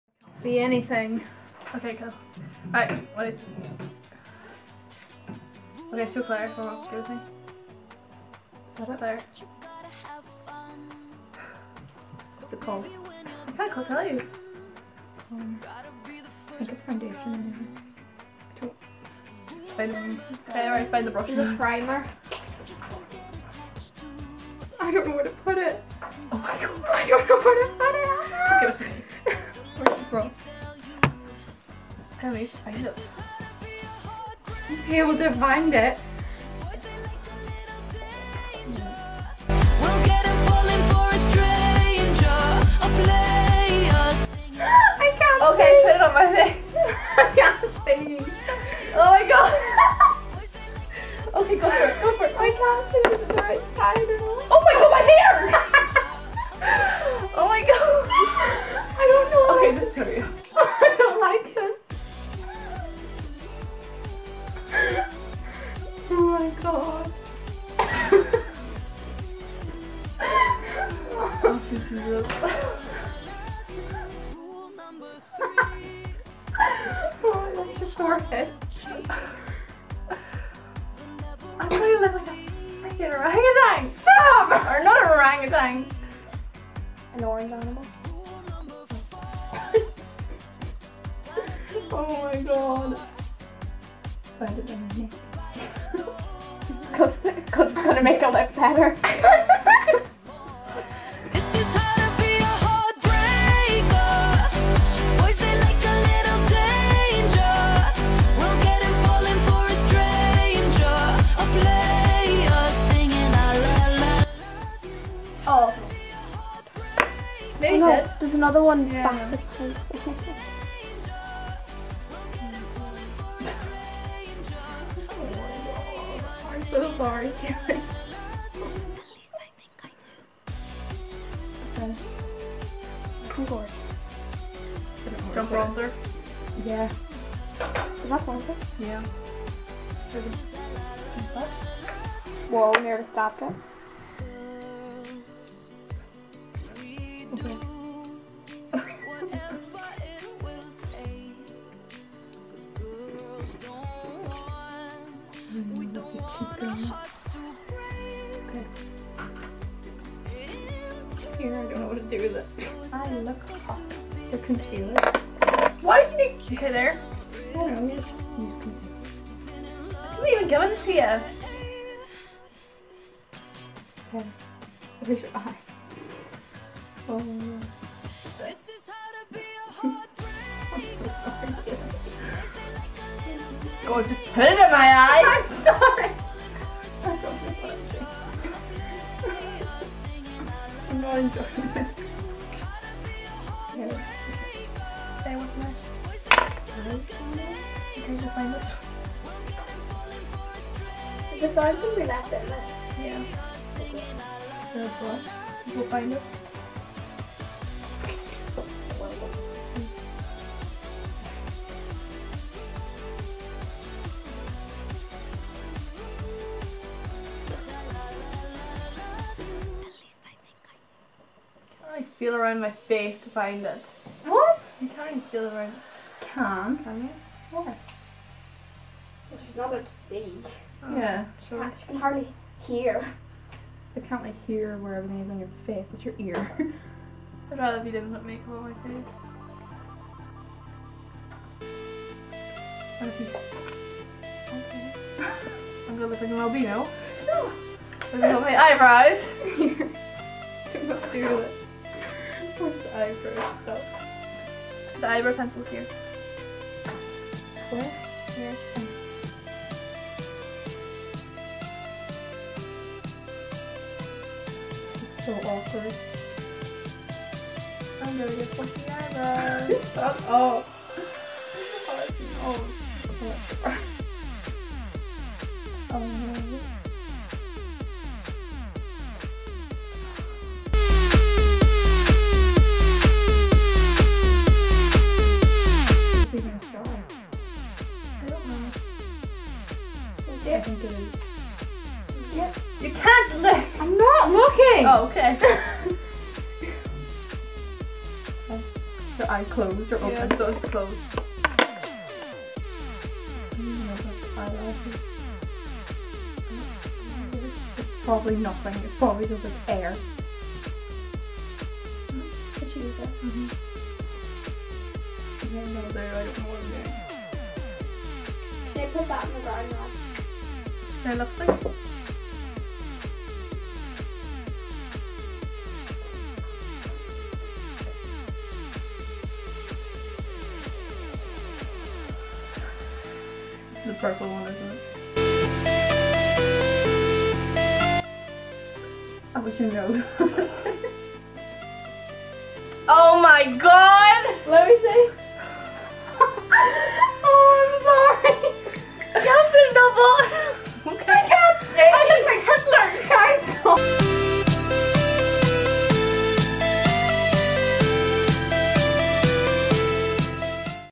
The Blindfolded Makeup Tag #1
So Sinéad and I got bored so we decided to do this for the craic, yep. Please excuse my no makeup-ness...

Products used (in order) :
- Maybelline Dream Satin Liquid (Ivory 010)
- Rimmel London Stay Matte Pressed Powder (Silky Beige 005)
- Mac Mineralize Skin Finish Natural (Medium/Dark)
- I'm not sure on the concealer but I think it was by Rimmel and the lightest shade
- Boujois Eye Shadow (White Ice - I think!) I'm not sure.
- Catrice Cosmetics Eyebrow Pencil (Brow-n-eyed Peas 030)
- Mia Minerals Black Mascara
- Kate Moss Lipstick (Not sure on the shade but it was a very dark purple)

Brushes used in the video:
- Bobbi Brown Foundation Brush
- Make Up Store Brush #371 (Fluffy Press Powder Brush)
- Bobbi Brown Bronzer Brush
- Make Up Store Brush #108 (Blending eyeshadow brush)

@bitemyt0ngue - Sinéad
@__ghostsnstuff - Kayleigh (Me)